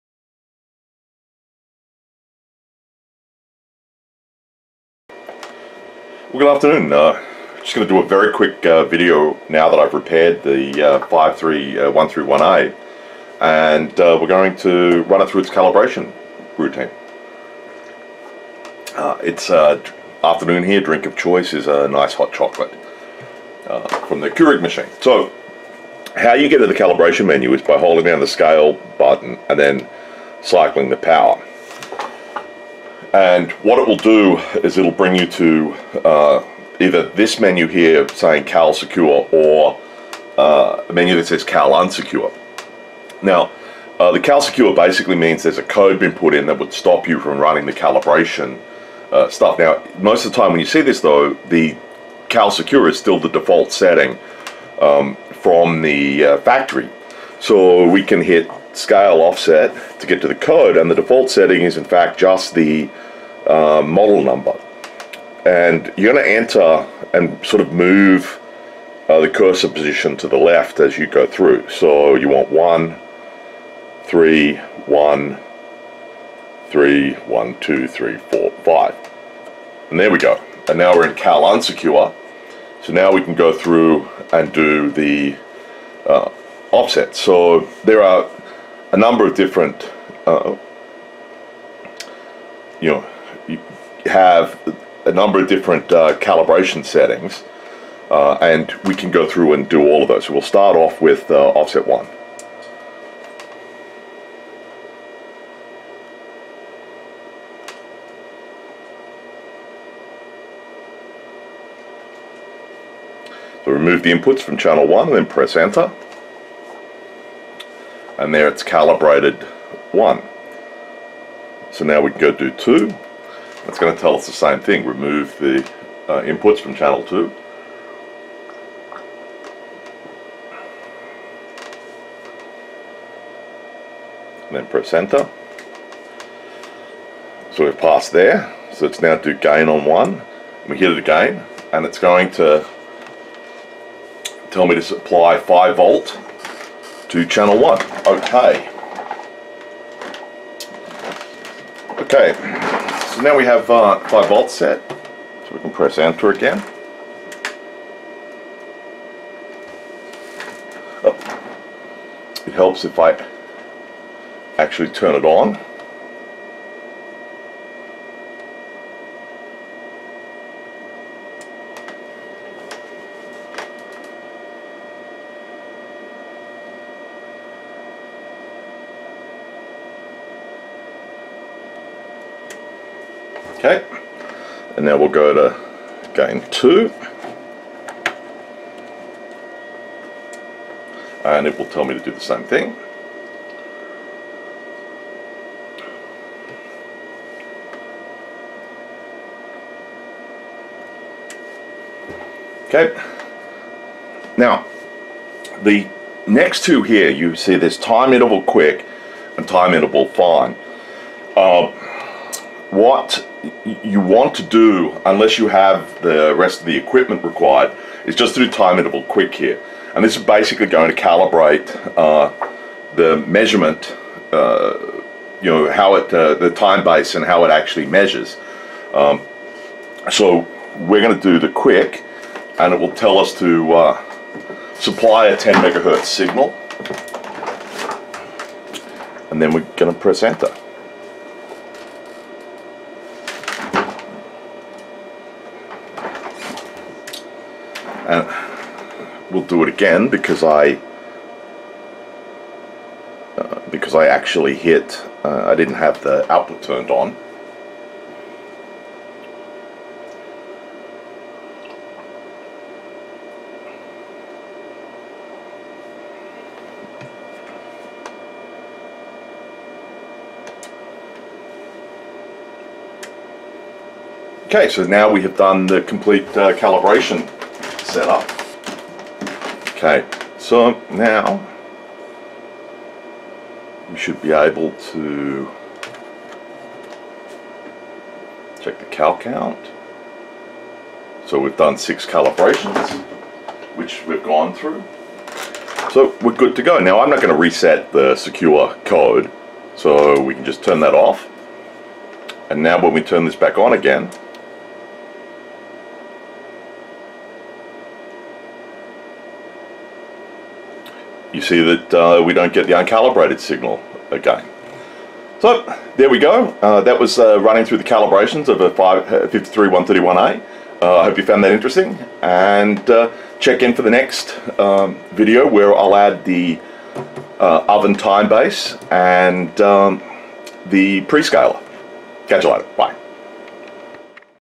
HP 53131A Calibration
This is a short video running through the various internal calibration options on the 53131A